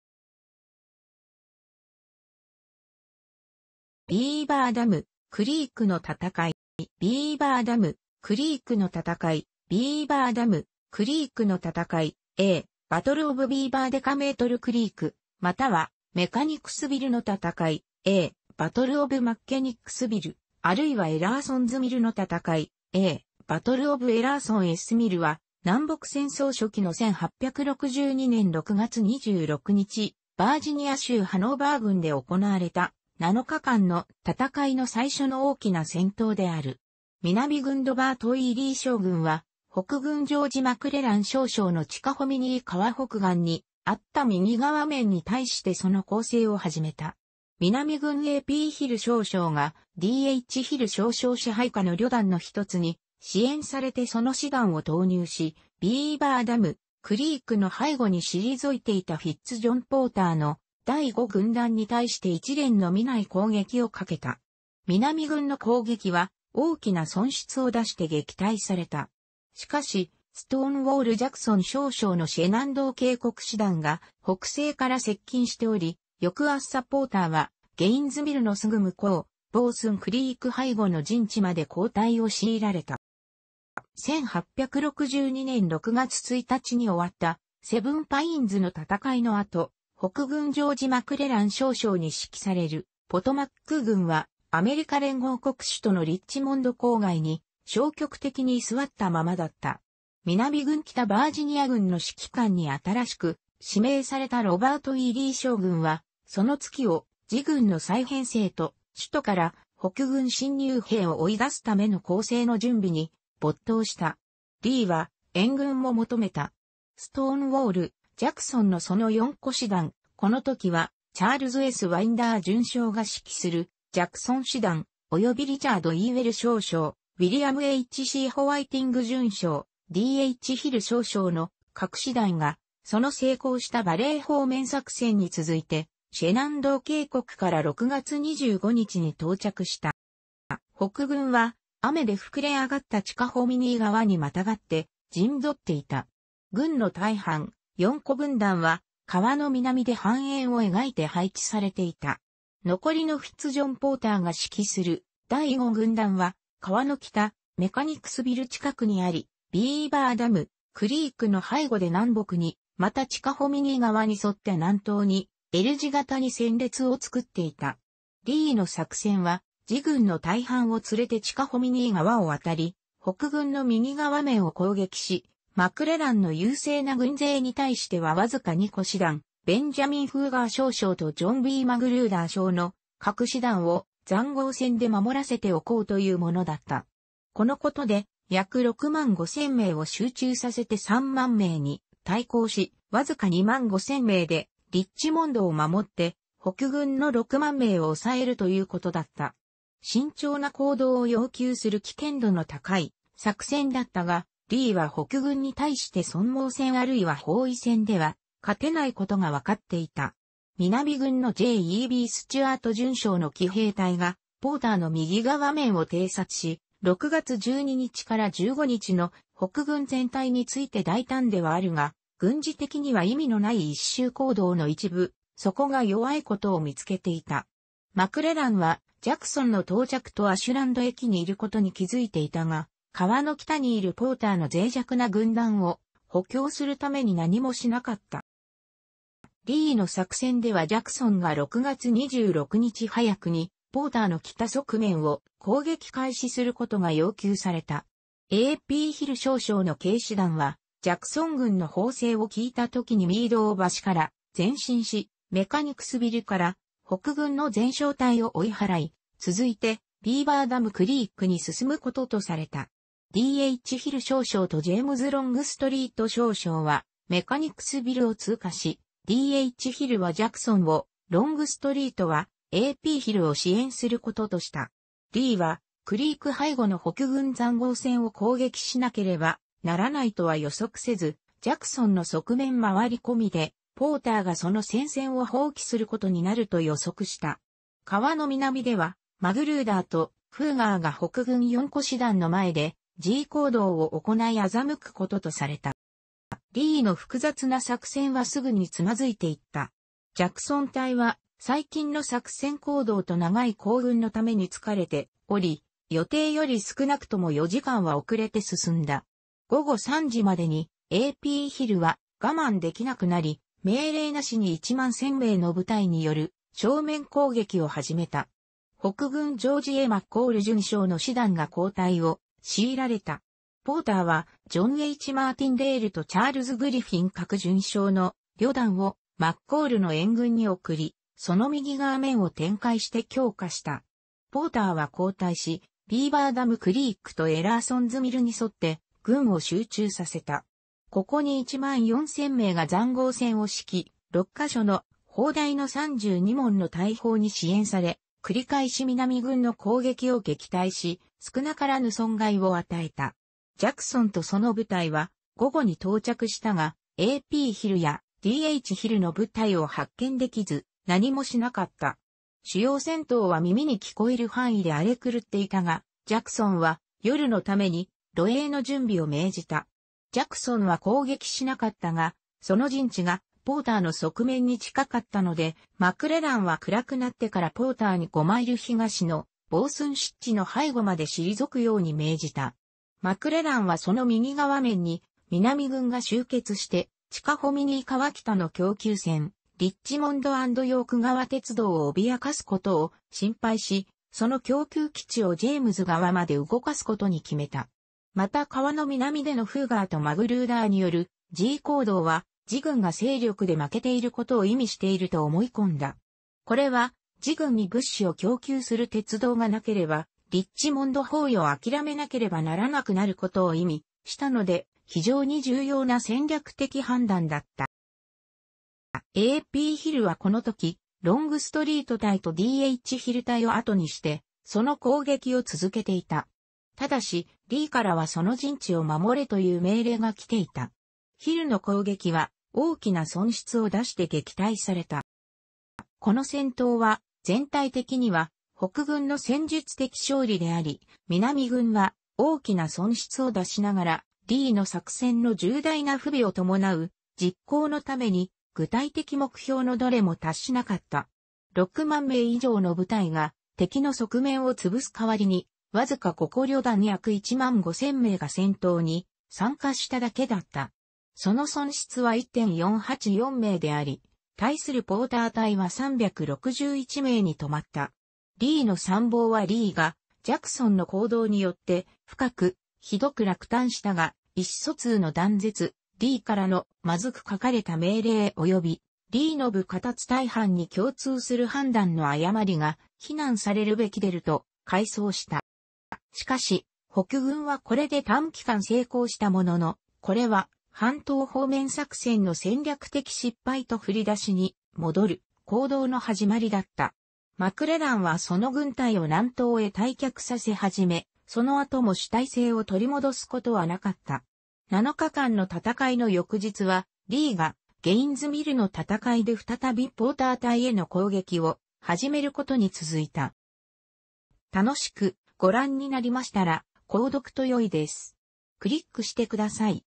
ビーバーダム・クリークの戦い
ビーバーダム・クリークの戦い(ビーバーダム・クリークのたたかい、英:Battle of Beaver Dam Creek、またはメカニクスビルの戦い、英:Battle of Mechanicsville、あるいはエラーソンズミルの戦い、英:Battle of Ellerson's Mill）は、南北戦争初期の1862年6月26日、バージニア州ハノーバー郡で行われた、七日間の戦いの最初の大きな戦闘である。南軍ロバート・E・リー将軍は、北軍ジョージ・マクレラン少将のチカホミニー川北岸にあった右側面に対してその攻勢を始めた。南軍A・P・ヒル少将がD・H・ヒル少将支配下の旅団の1つに支援されてその師団を投入し、ビーバーダム・クリークの背後に退いていたフィッツ・ジョン・ポーターの第5軍団に対して一連の実らない攻撃を掛けた。南軍の攻撃は大きな損失を出して撃退された。しかし、ストーンウォール・ジャクソン少将のシェナンドー渓谷師団が北西から接近しており、翌朝ポーターはゲインズミルの直ぐ向こう、ボースン・クリーク背後の陣地まで後退を強いられた。

1862年6月1日に終わったセブンパインズの戦いの後、北軍ジョージ・マクレラン少将に指揮されるポトマック軍はアメリカ連合国首都のリッチモンド郊外に消極的に居座ったままだった。南軍北バージニア軍の指揮官に新しく指名されたロバート・E・リー将軍は、その月を自軍の再編成と首都から北軍侵入兵を追い出すための攻勢の準備に没頭した。リーは援軍も求めた。ストーンウォール・ジャクソンのその4個師団（この時はチャールズ・S・ワインダー准将が指揮するジャクソン師団、およびリチャード・イーウェル少将、ウィリアム・H・C・ホワイティング准将、D・H・ヒル少将の各師団）がその成功したバレー方面作戦に続いてシェナンドー渓谷から6月25日に到着した。

北軍は雨で膨れ上がったチカホミニー川に跨って陣取っていた。軍の大半、4個軍団は川の南で半円を描いて配置されていた。残りのフィッツ・ジョン・ポーターが指揮する第5軍団は川の北、メカニクスビル近くにあり、ビーバーダム・クリークの背後で南北に、またチカホミニー川に沿って南東にL字形に戦列を作っていた。リーの作戦は自軍の大半を連れてチカホミニー川を渉り、北軍の右側面を攻撃し、マクレランの優勢な軍勢に対してはわずか2個師団（ベンジャミン・フーガー少将とジョン・B・マグルーダー少将の各師団）を塹壕線で守らせておこうというものだった。このことで約65,000名を集中させて30,000名に対抗し、わずか25,000名でリッチモンドを守って、北軍の60,000名を抑えるということだった。慎重な行動を要求する危険度の高い作戦だったが、リーは北軍に対して損耗戦あるいは包囲戦では勝てないことが分かっていた。南軍のJ・E・B・スチュアート准将の騎兵隊がポーターの右側面を偵察し（6月12日から15日の北軍全体について大胆ではあるが軍事的には意味の無い一周行動の一部）、そこが弱いことを見つけていた。マクレランはジャクソンの到着とアシュランド駅にいることに気付いていたが、川の北に居るポーターの脆弱な軍団を補強するために何もしなかった。

リーの作戦ではジャクソンが6月26日早くにポーターの北側面を攻撃開始することが要求された。A・P・ヒル少将の軽師団はジャクソン軍の砲声を聞いたときにミードウ橋から前進し、メカニクスビルから北軍の前哨隊を追い払い、続いてビーバーダム・クリークに進むこととされた。D・H・ヒル少将とジェイムズ・ロングストリート少将はメカニクスビルを通過し、D・H・ヒルはジャクソンを、ロングストリートはA・P・ヒルを支援することとした。リーはクリーク背後の北軍塹壕線を攻撃しなければならないとは予測せず、ジャクソンの側面回りこみでポーターがその戦線を放棄することになると予測した。川の南では、マグルーダーとフーガーが北軍4個師団の前で示威行動を行い欺くこととされた。

リーの複雑な作戦は直ぐに躓いていった。ジャクソン隊は最近の作戦行動と長い行軍のために疲れており、予定より少なくとも4時間は遅れて進んだ。午後3時までにA・P・ヒルは我慢できなくなり、命令無しに11,000名の部隊による正面攻撃を始めた。北軍ジョージ・A・マッコール准将の師団が後退を強いられた。ポーターはジョン・H・マーティンデールとチャールズ・グ...